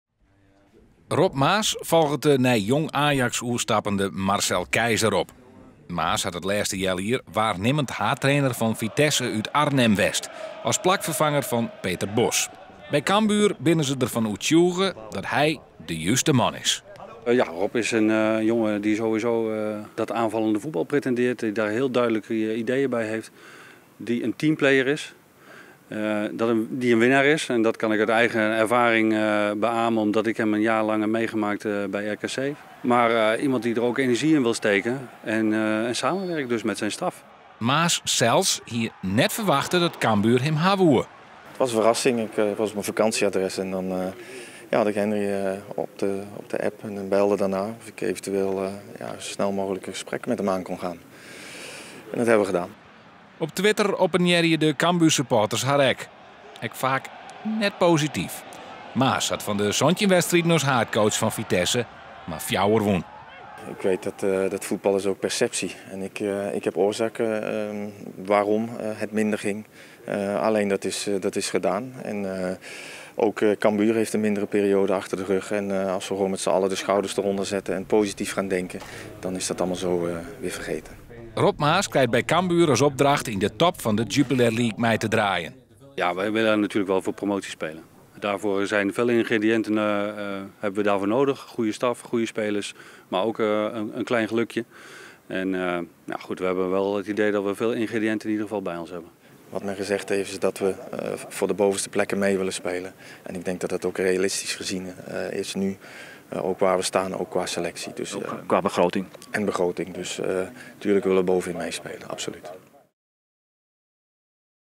Rob Maas is blij met 'warme, hardwerkende volksclub' SC Cambuur
Rob Maas had zelf niet verwacht dat SC Cambuur hem wilde hebben, na het slechte laatste halfjaar dat hij had bij Vitesse. Dat zei hij vrijdag tijdens de persconferentie bij de Leeuwarder club. De nieuwe hoofdtrainer is blij met de overstap. Hij noemt Cambuur een warme volksclub, waar hard werken centraal staat.

Ook Henry van der Vegt van Cambuur is opgetogen. Rob Maas voldoet aan het profiel van aanvallend spelen, verzorgd voetbal en is een teamplayer, aldus de technisch directeur. Maas komt voor een seizoen naar Cambuur, met de optie op nog een seizoen.